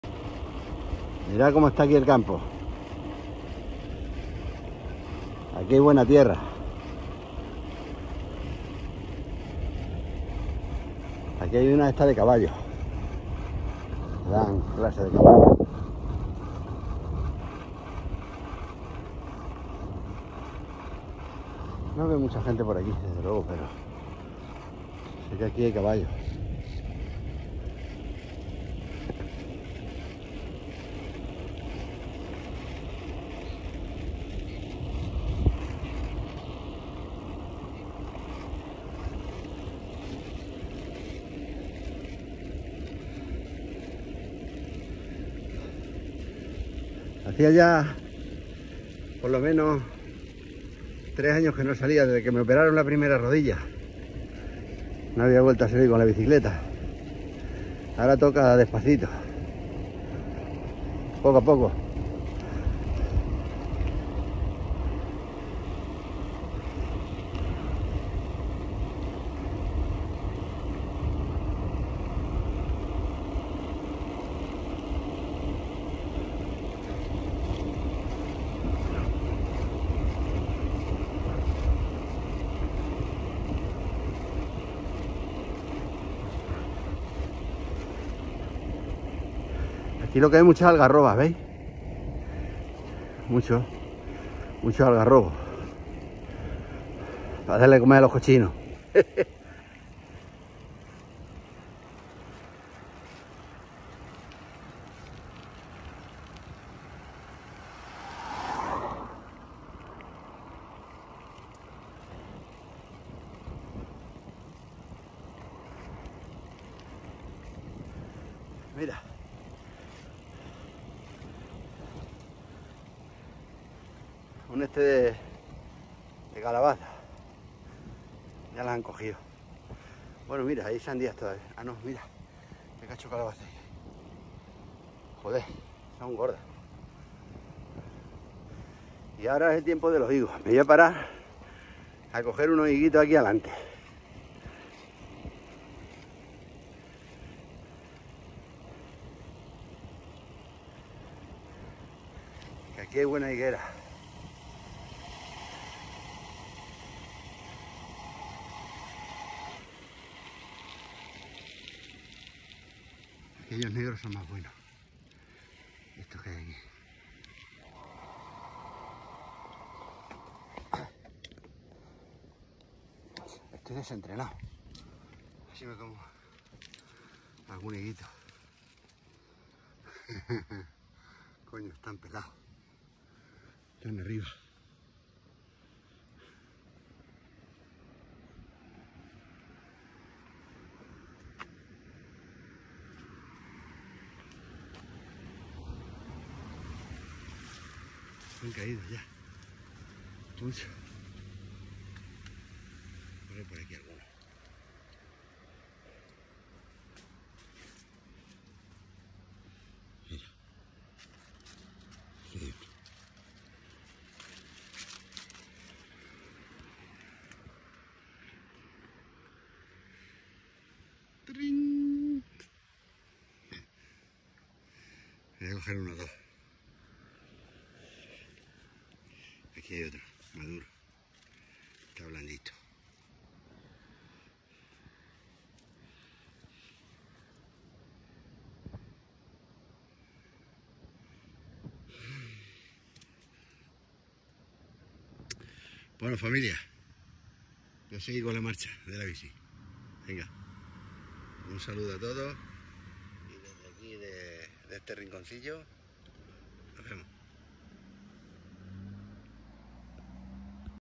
salida en bicicleta por la isla de ibiza
amigos despues de tres años vuelvo a salir gracias a todos por buestro apoyo